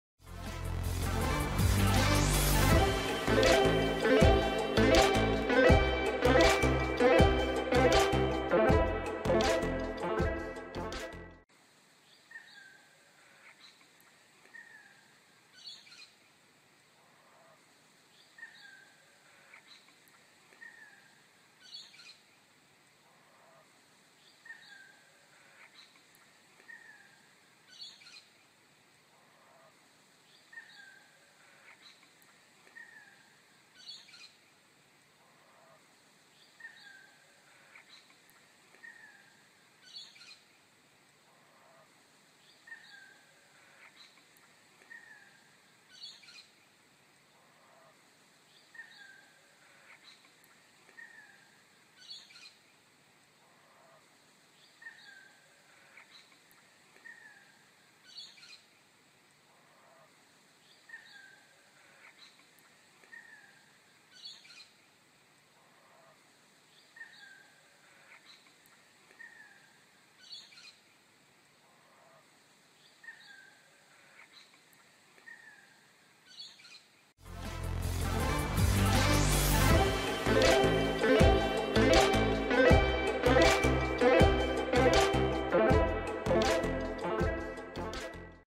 UFO Sighting or Camera Flare? You Decide
22 June 2019 - Was down the back paddock with my cat when i noticed this object flying around.
I managed to catch just a few seconds before it just suddenly vanished.
Can you see it? Look to the right of the sun.
What do you think it could have been?